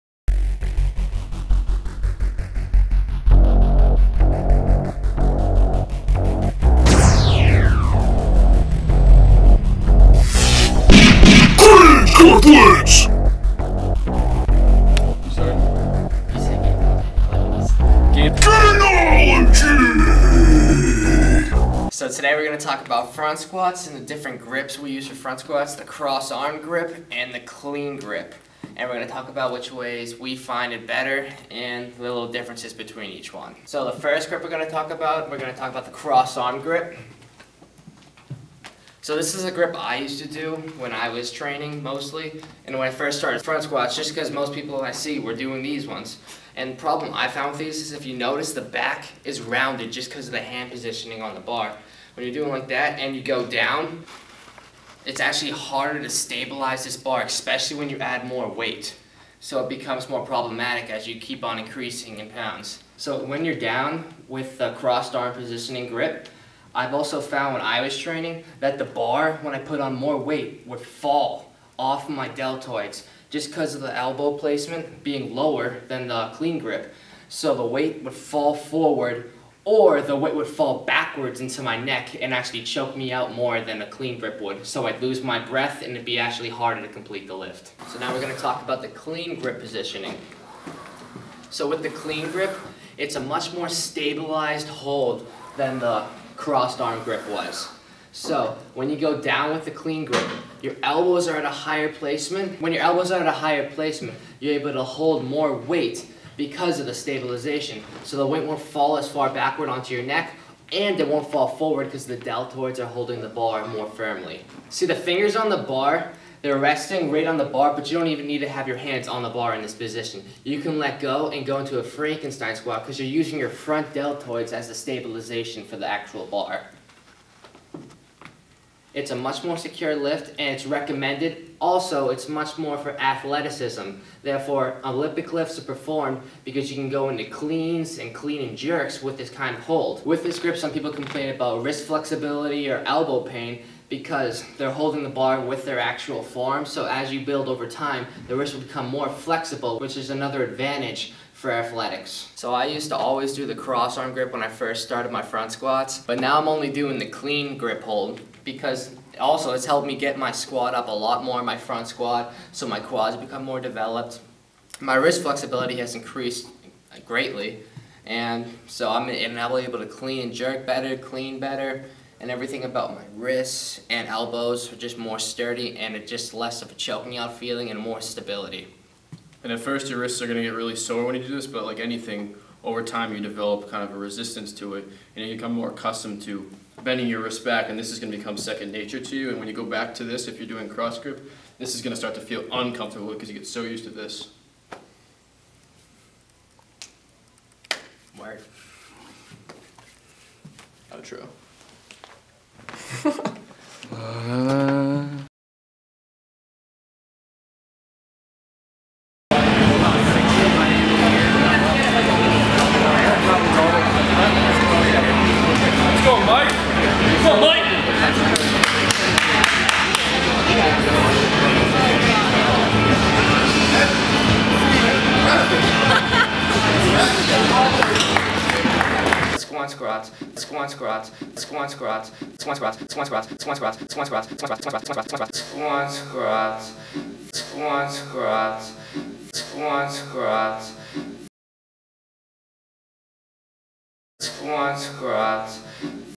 Front Squats: Crossed-Arm Grip vs. Clean Grip
In this video we discuss the differences between the two different front squat grips: Crossed-Arm Grip and Clean Grip. There are advantages and disadvantages to both grips, but we discuss more thoroughly the clean grip for it is the more preferred method. Also, we personally recommend the clean grip for front squats because we've found more success and gains using this grip as well as beneficial assistance for other lifts like the clean and the clean and jerk along with overall athleticism. If front squats are not already in your workout program, we would also highly recommend you add some form of them to your program as a variation from the back squat and to put much more emphasis on the quadriceps. Get some Gainology on those leg days Girthlings!!